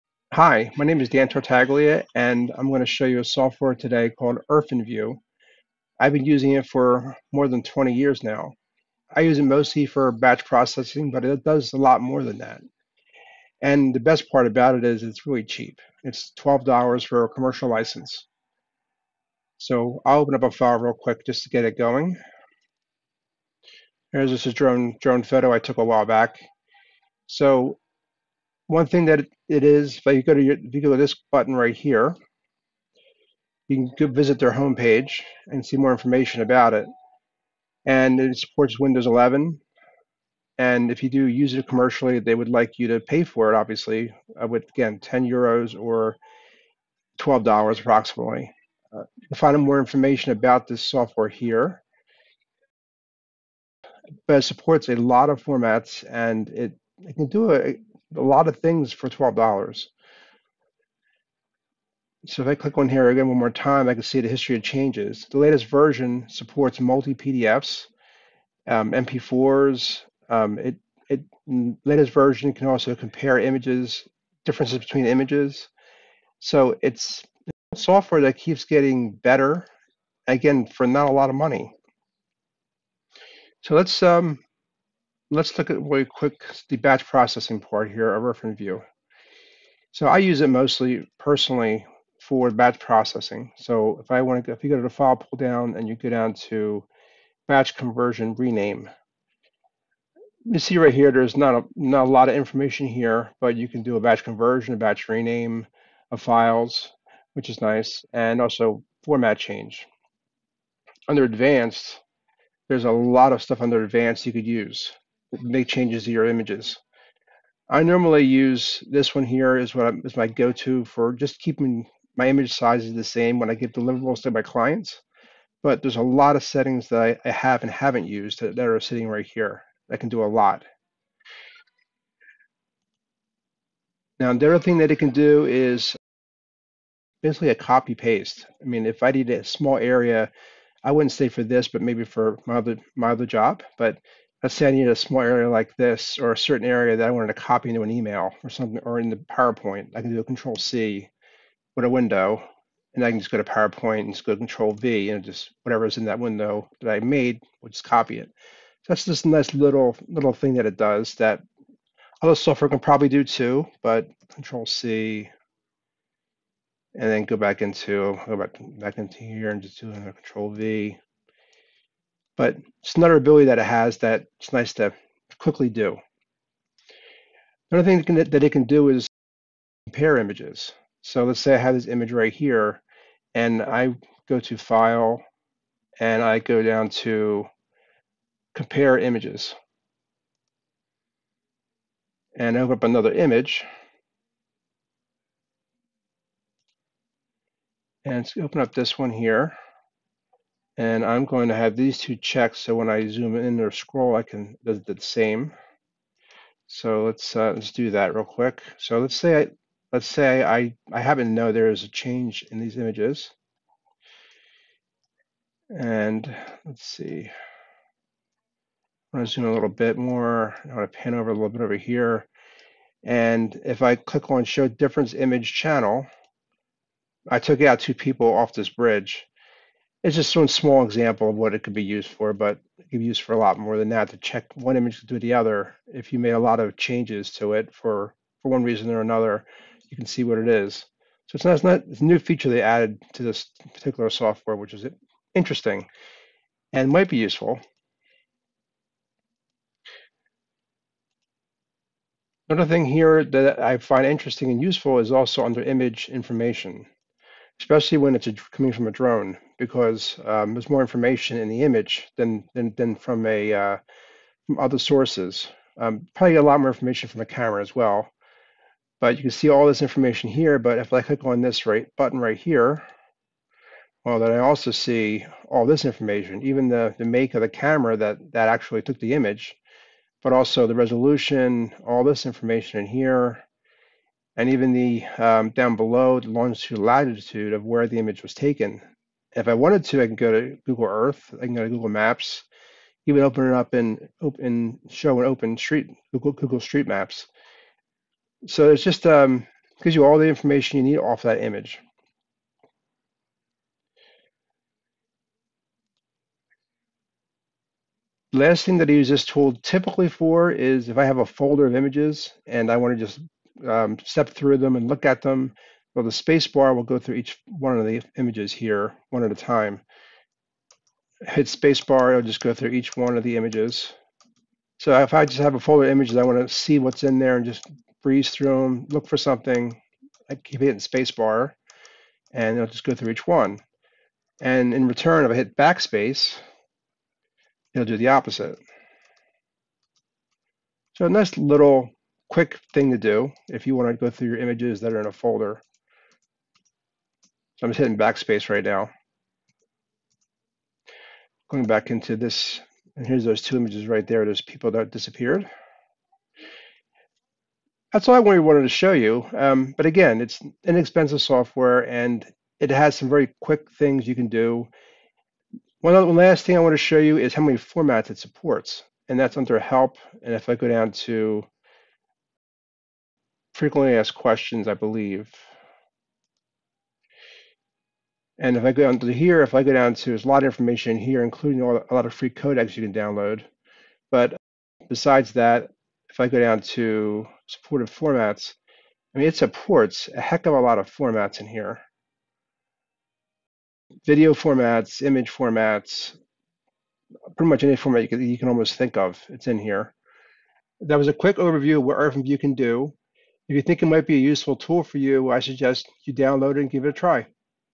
Introduction to the IrfanView Graphic Viewer
I've been using this software for many years and thought to myself why I haven't posted a video to support it's use.

Sorry for the background or my energy level, recorded it at 1:00am :-(

Chapters:
0:00 Introduction
0:31 Home webpage
0:41 How to pay for it
0:49 A few of it's abilities
1:24 Batch processing
2:16 Quick copy/paste
2:58 Compare images
4:12 Image information
5:08 Quickly view images in a folder
6:05 Free codecs & many supported formats
6:40 Ending